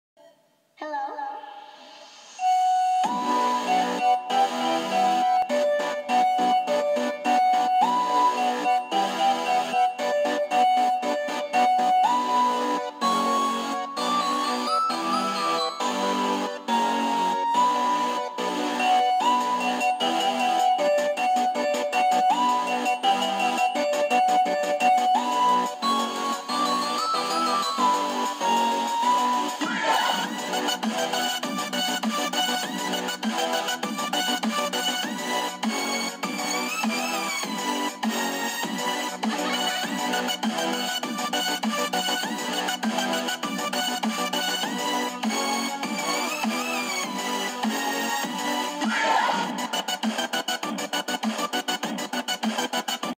Update for my channel
I have cancelled my GLMV do do other stuff so yeah :/